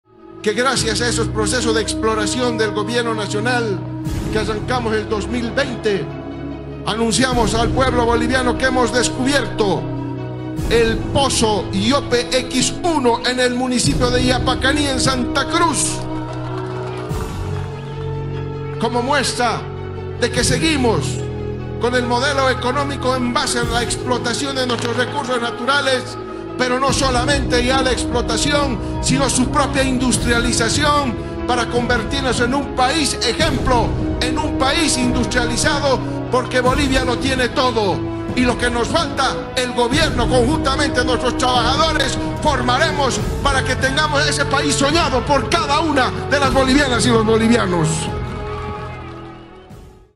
¡El descubrimiento del Pozo Yope-X1 permitirá una producción de más gas para Bolivia!
El descubrimiento del Pozo Yope-X1 en Yapacaní, SantaCruz, permitirá una producción de gas de más de 1,4 millones de pies cúbicos/día y 115 barriles/día de condensado. Este es el resultado de un proceso de exploración responsable.
¡La reconstrucción de la Patria🇧🇴 no se detiene!